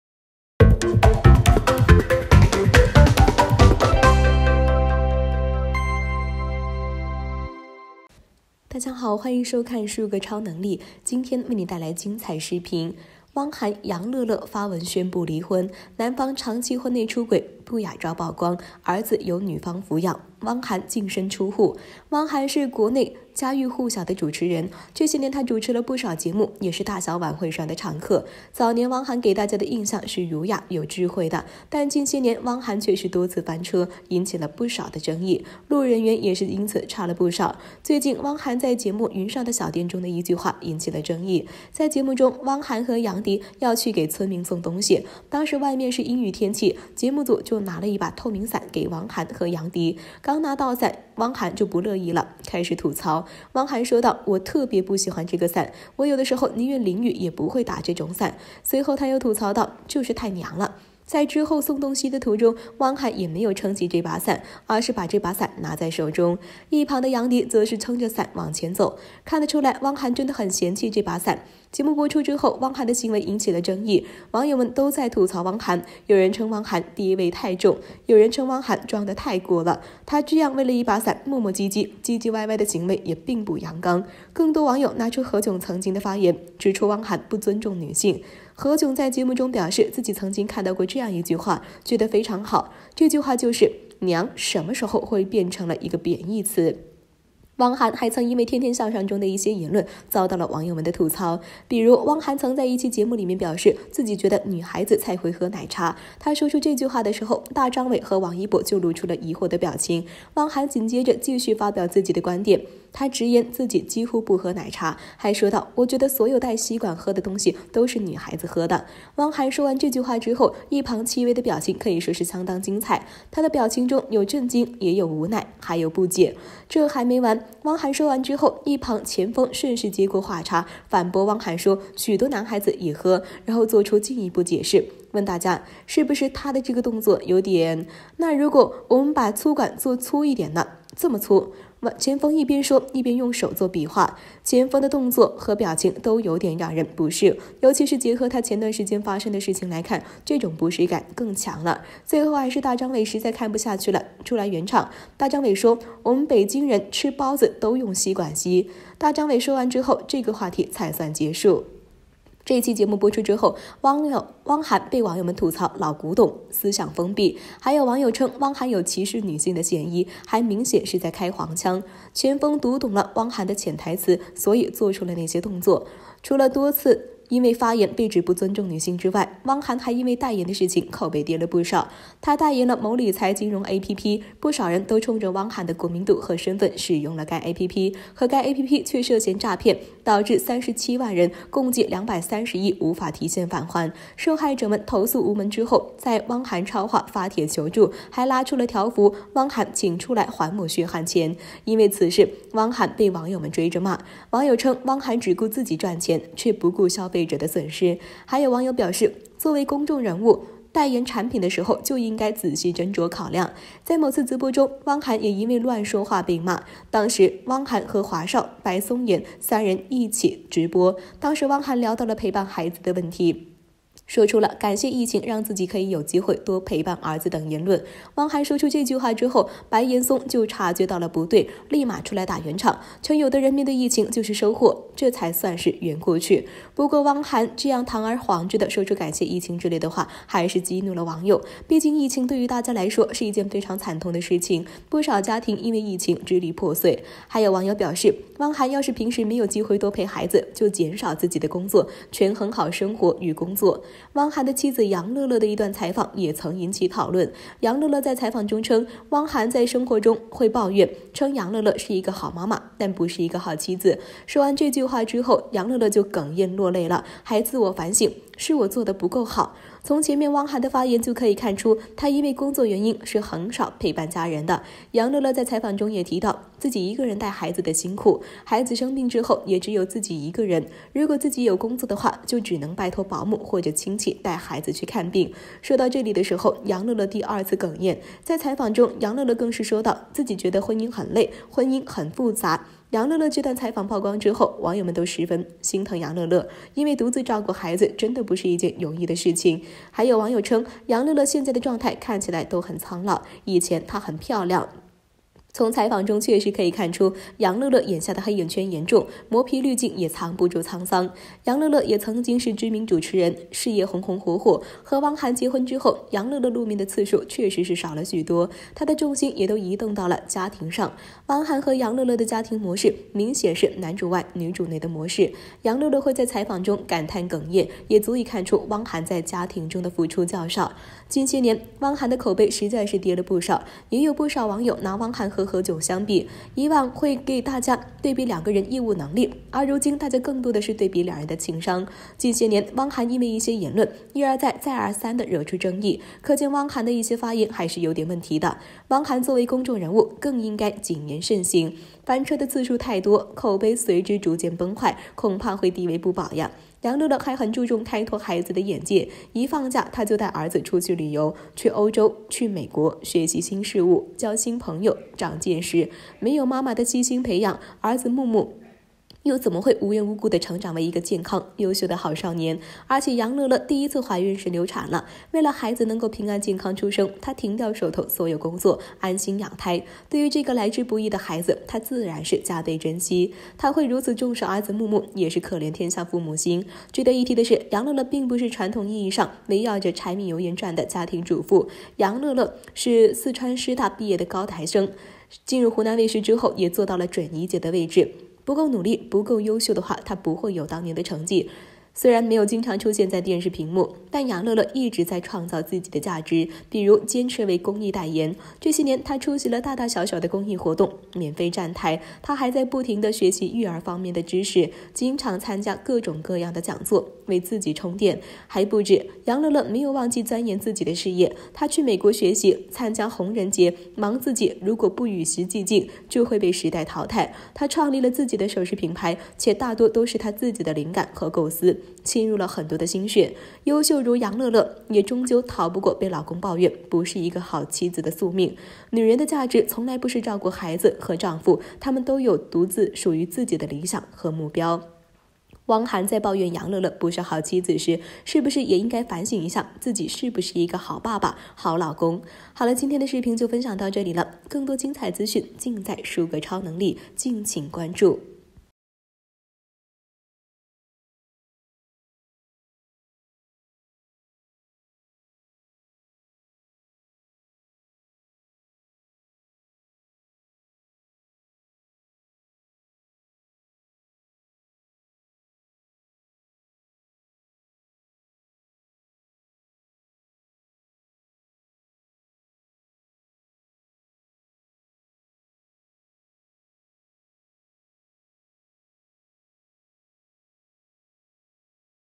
汪涵楊樂樂發文宣布離婚！男方長期婚內出軌不雅照曝光，兒子由女方撫養，汪涵身出戶！數個超能力
汪涵楊樂樂發文宣布離婚！男方長期婚內出軌不雅照曝光，兒子由女方撫養，汪涵身出戶！數個超能力#汪涵#楊樂樂